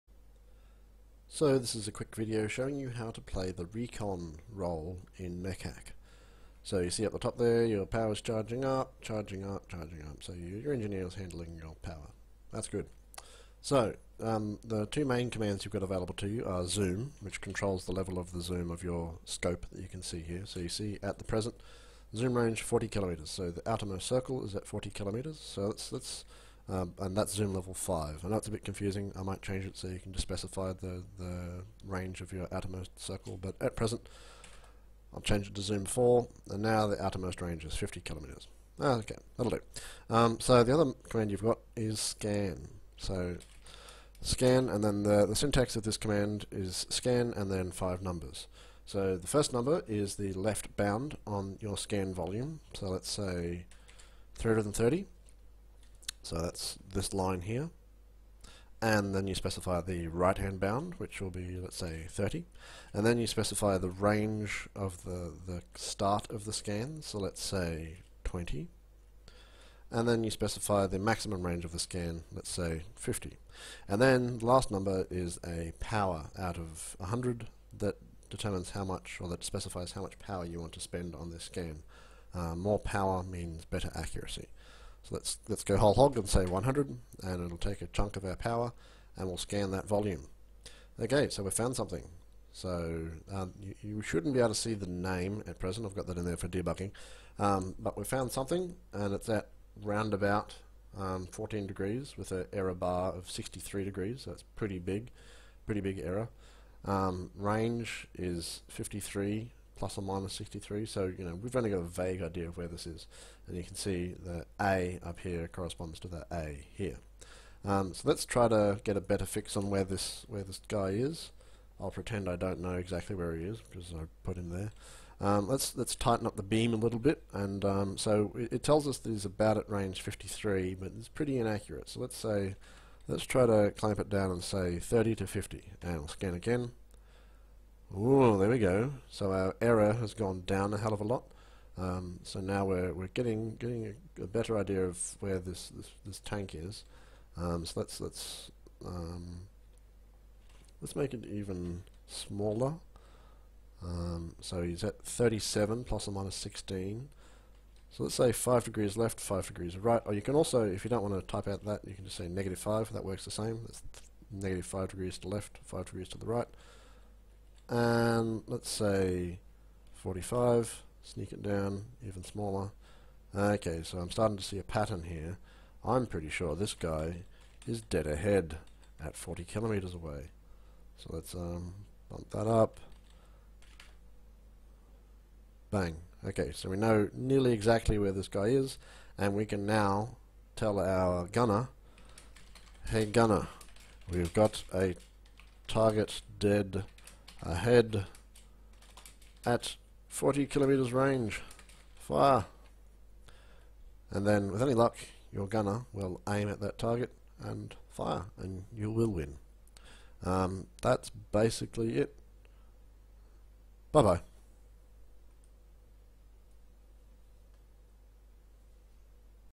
Mechack video #9 - How to play Recon
A quick guide on how to play the recon role.